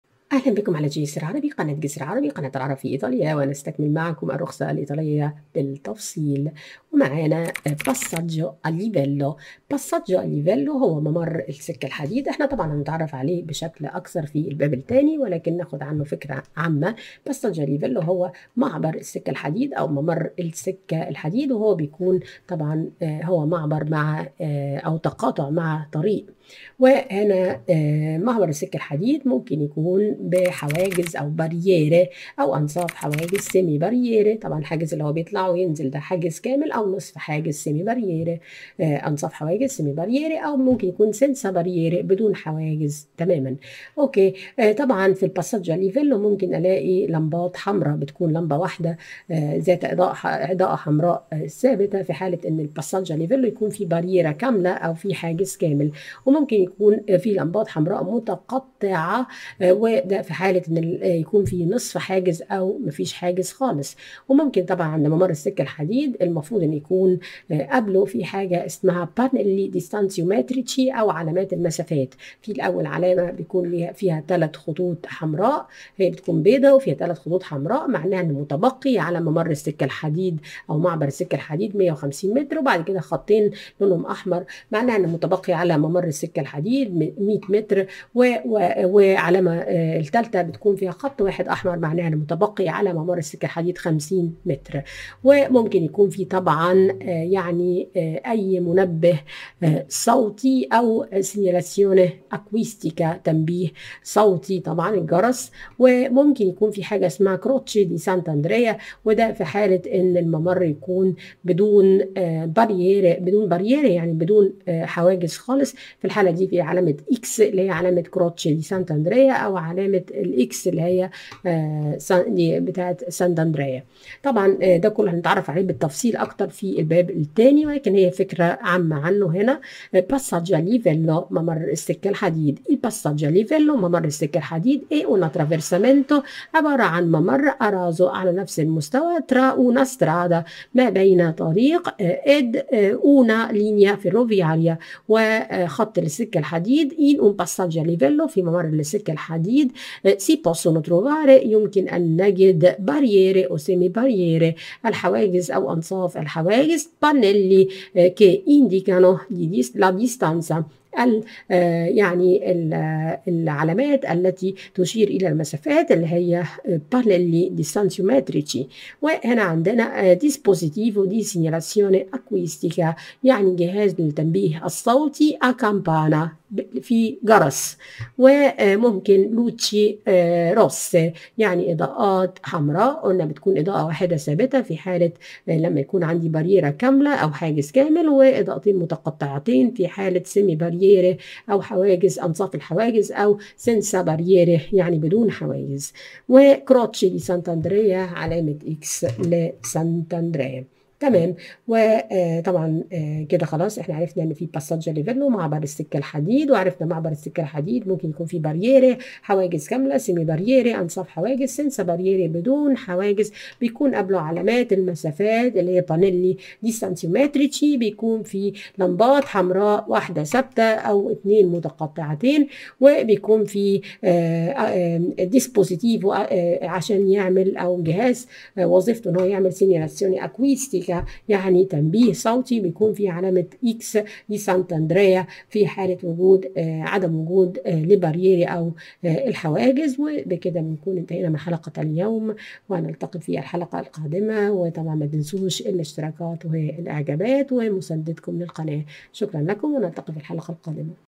16 Passaggio a livello رخصة القيادة الإيطالية
16 Passaggio a livello
ممر السكة الحديد
شرح تفصيلي شامل لجميع أجزاء الرخصة الإيطالية مقدم للعرب المقيمين في إيطاليا.